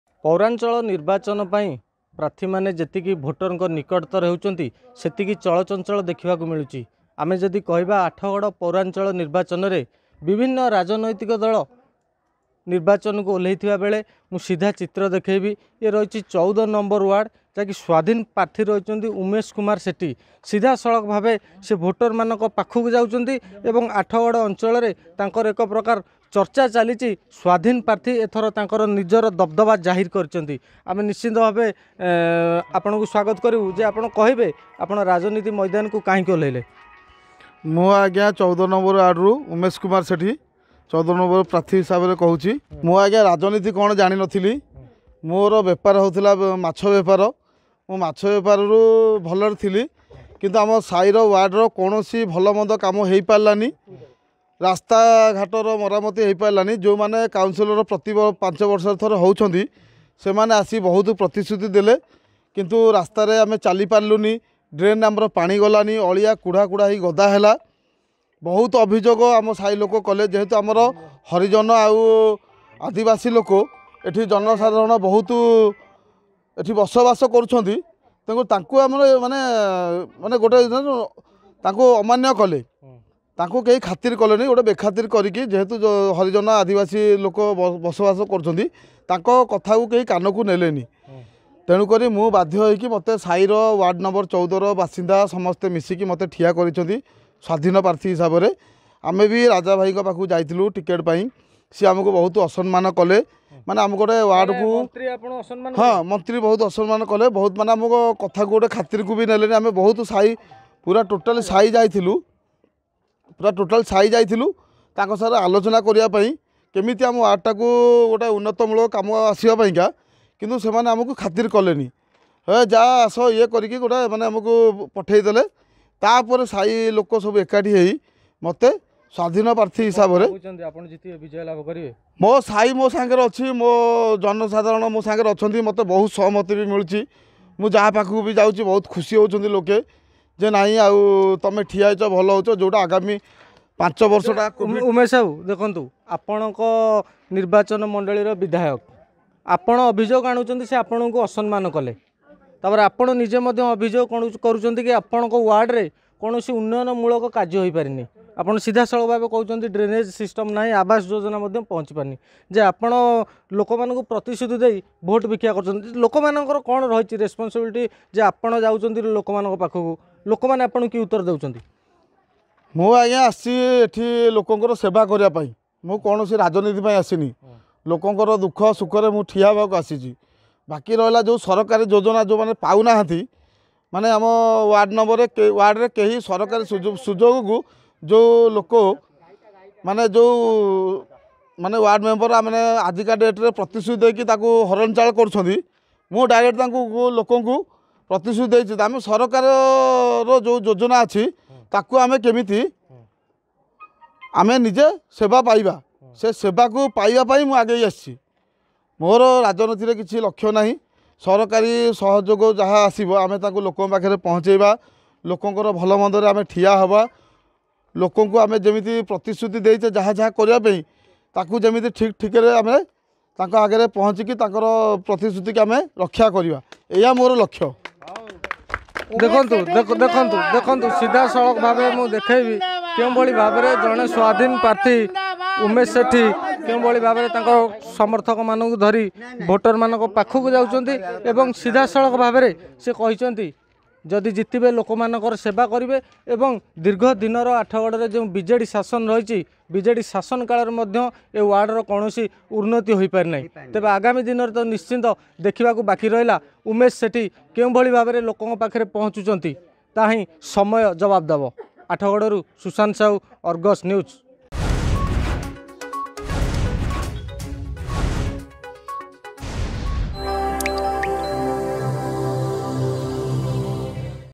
Odisha Municipal Elections: Umesh Kumar Sethi Campaigning Alone Against BJD in Athagarh
Odisha Municipal Elections: Umesh Kumar Sethi Campaigning Alone As Independent Against BJD in Athagarh
ରାଜା ବାବୁ ଅସମ୍ମାନ କଲେ, ତେଣୁ ସ୍ଵାଧିନ ଭାବେ ଲଢିବି ଏବଂ ଜିତିବି । ଆମ ୱାର୍ଡ଼ ରେ କୌଣସି ବିକାଶ ହେଇନି । ଏଭଳି କହିଛନ୍ତି ସ୍ଵାଧିନ କାଉନସିଲର ପ୍ରାର୍ଥି ଉମେଶ କୁମାର ସେଠୀ